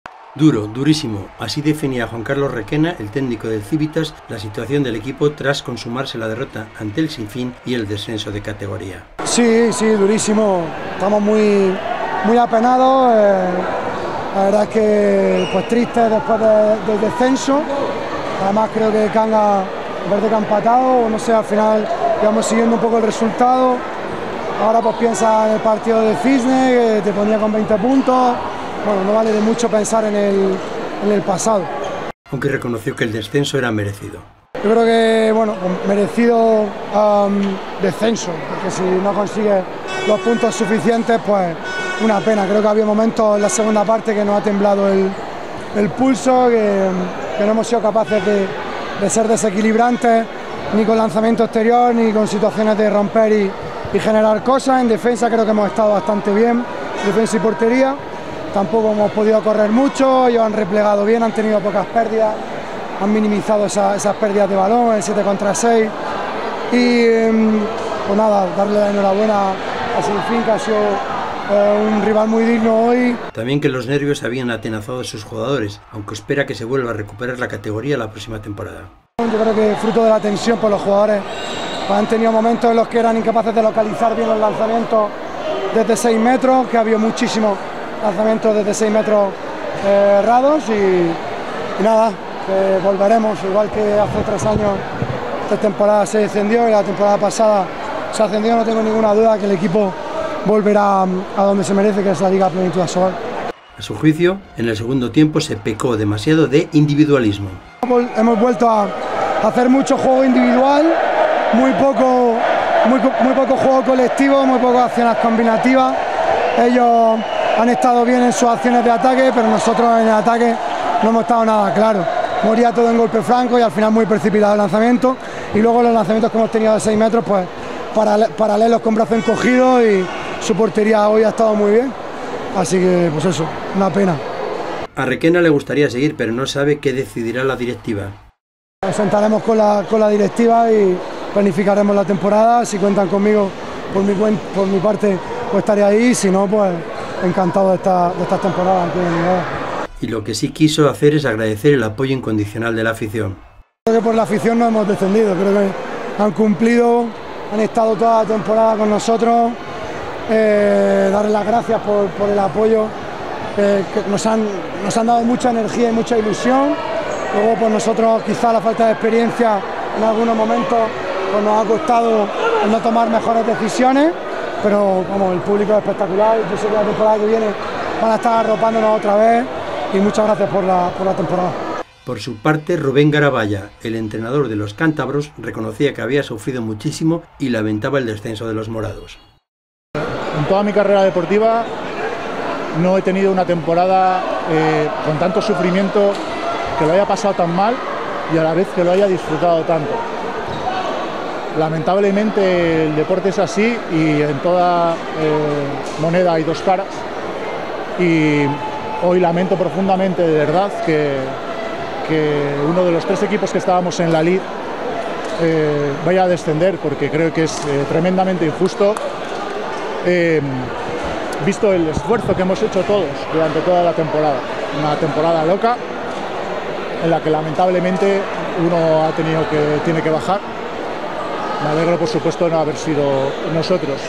Requena lamenta que en el segundo tiempo hubo errores que les llevaron a la derrota
El técnico morado no sabe si continuará la próxima temporada, aunque le gustaría, y agradeció el poyo total e incondicional de la afición. Cree que el año próxima se estará celebrando de nuevo el ascenso a la Asobal